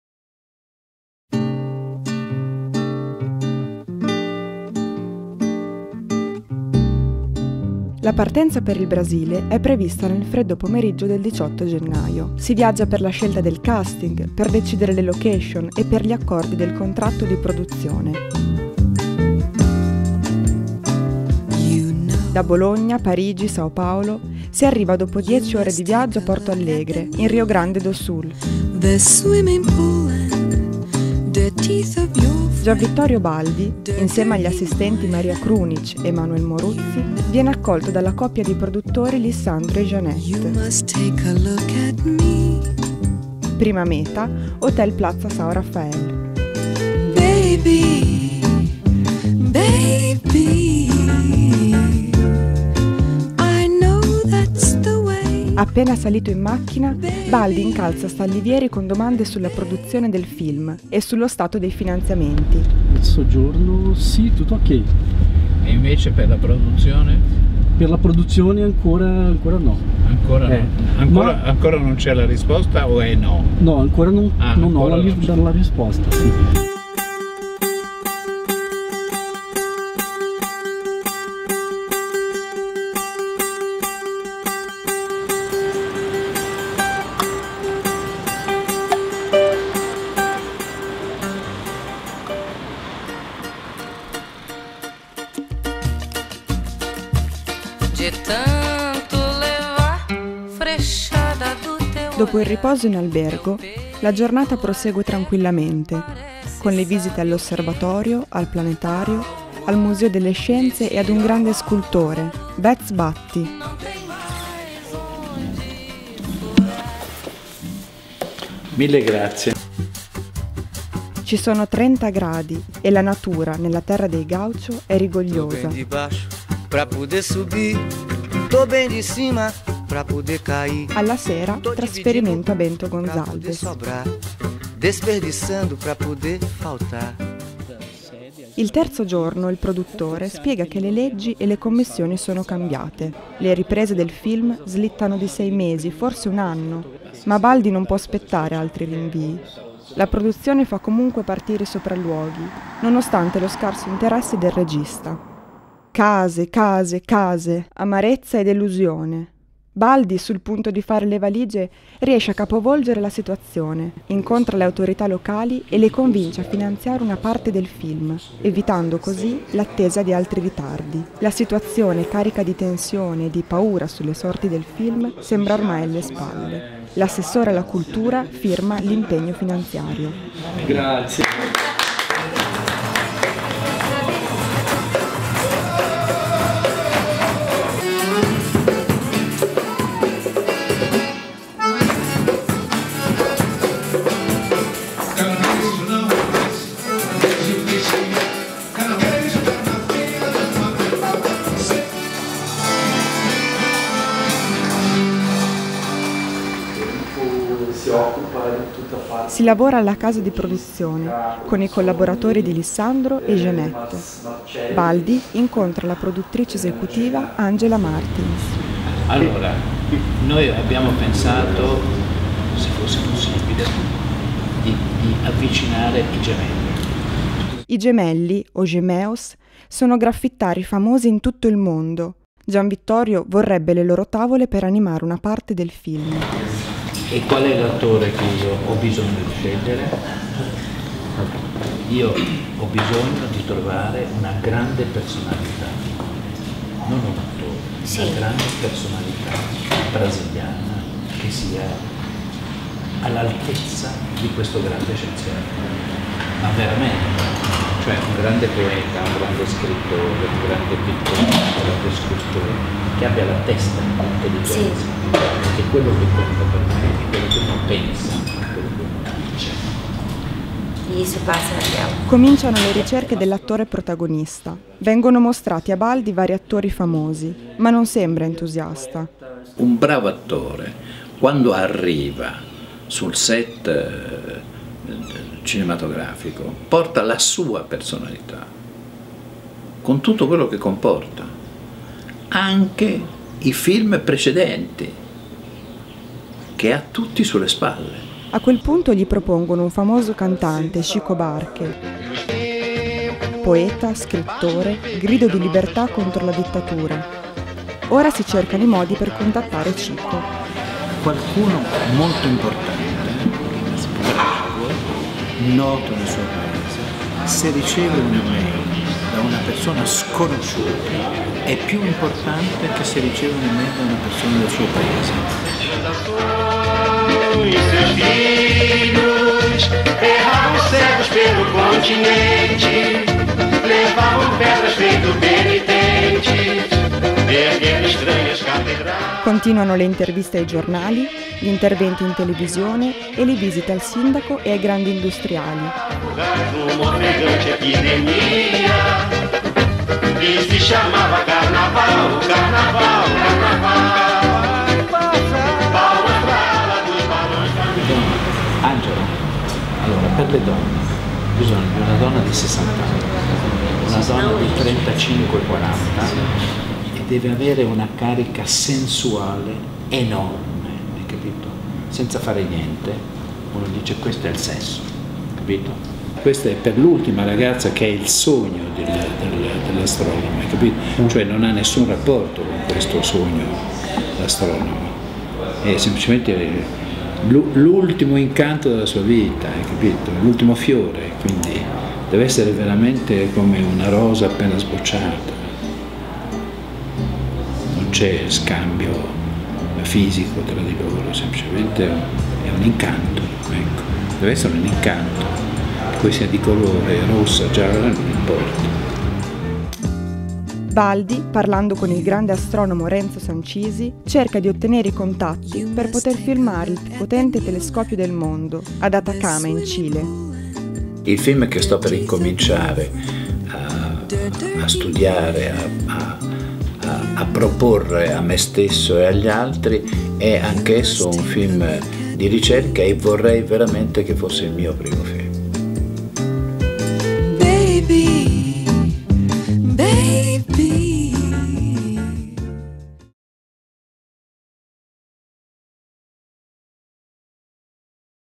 Il Cielo sopra di Me - Pre-produzione #1: Diario di viaggio in Brasile
Viaggio in Brasile - 18/30 Gennaio 2011

Gian Vittorio Baldi va in Brasile per effettuare i sopralluoghi, casting e continuare la pre-produzione del suo prossimo film, "Il cielo sopra di me".